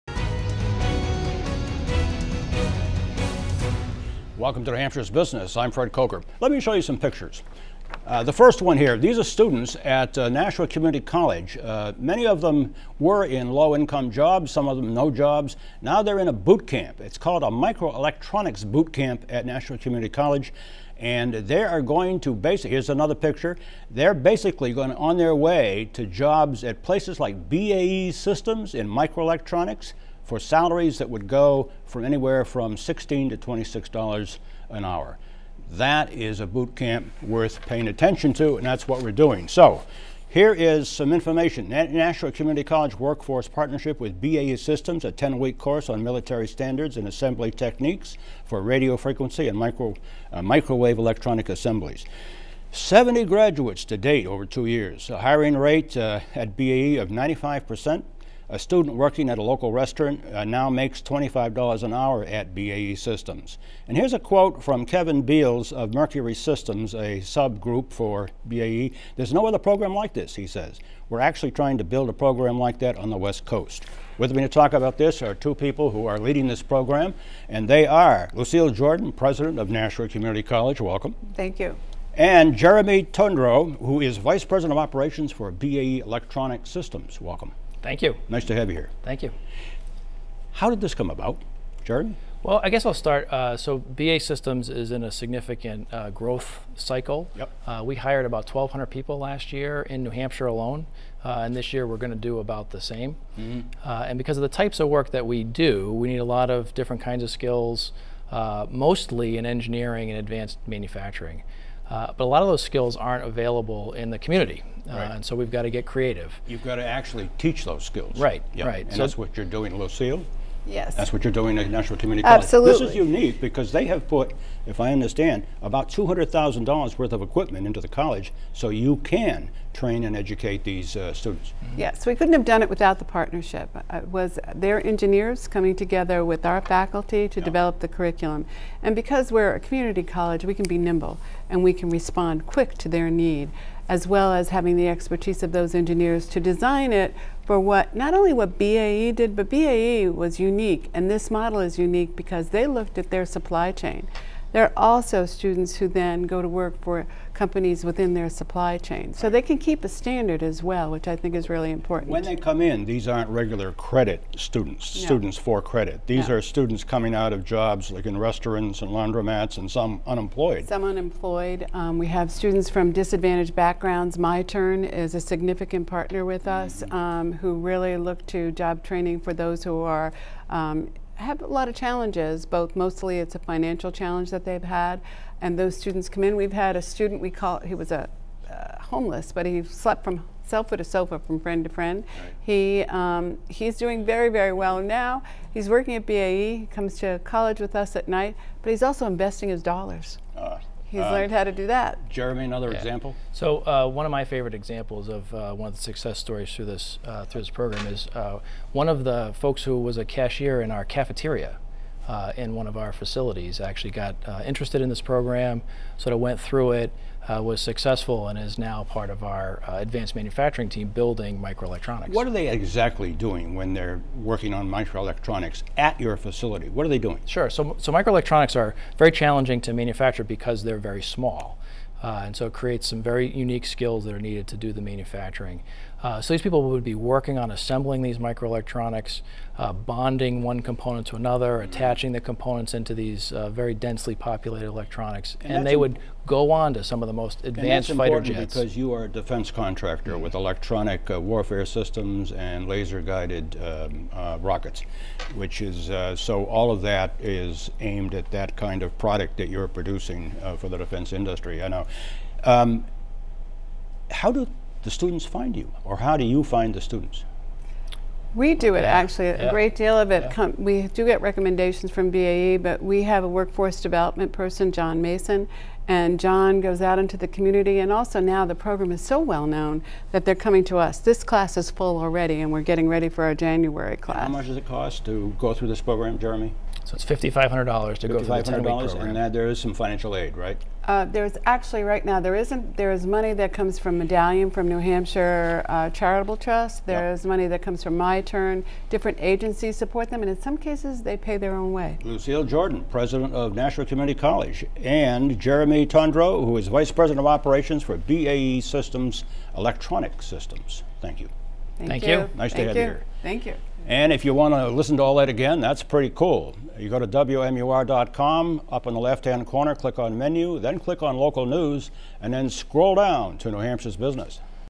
NH Bootcamp - from low paying jobs to $25/hour microelectronics jobs in 10 weeks
Fred Kocher sits down with Jeremy Tondreault of BAE Electronic Systems and Nashua Community College President Lucille Jordan to discuss how workers at low-paying jobs can transition to higher-paying jobs with ten weeks of training